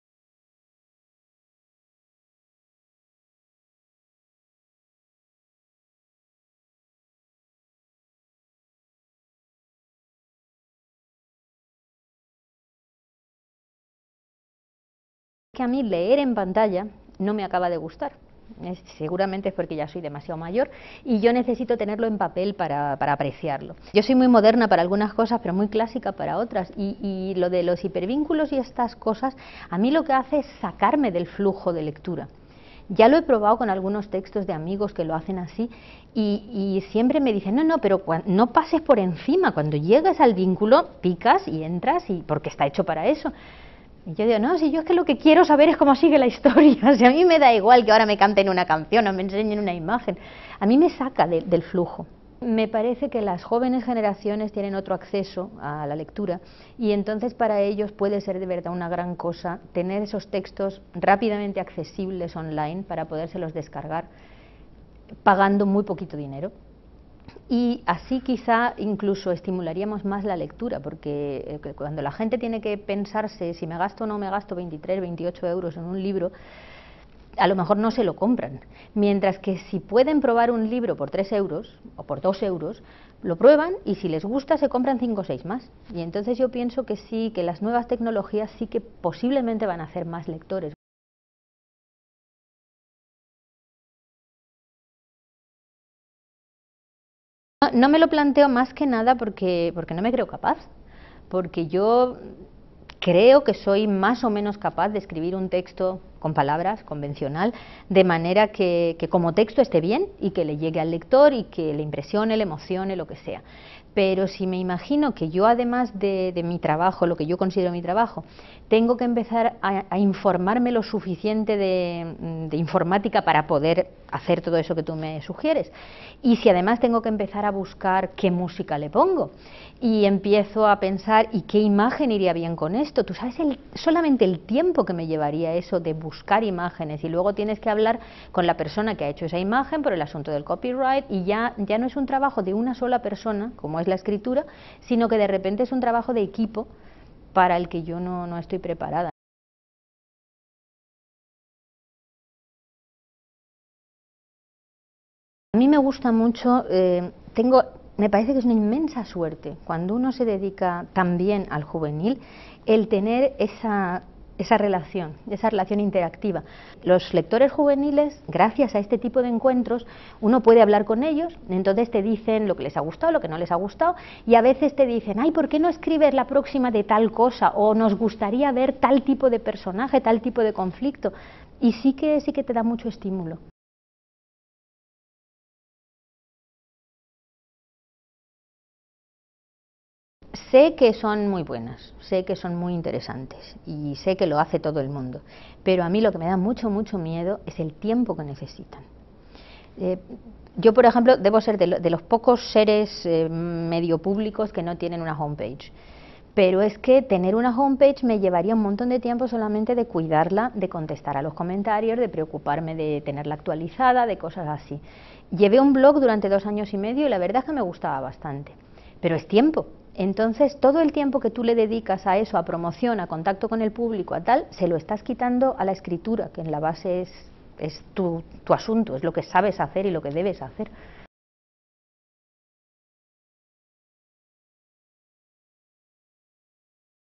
Entrevista a Elia Barelo
Entrevista a la escritora Elia Barceló en el encuentro con los lectores que realizó en el Centro Internacional del Libro Infantil y Juvenil (CILIJ) de la Fundación Germán Sánchez Ruipérez (FGSR).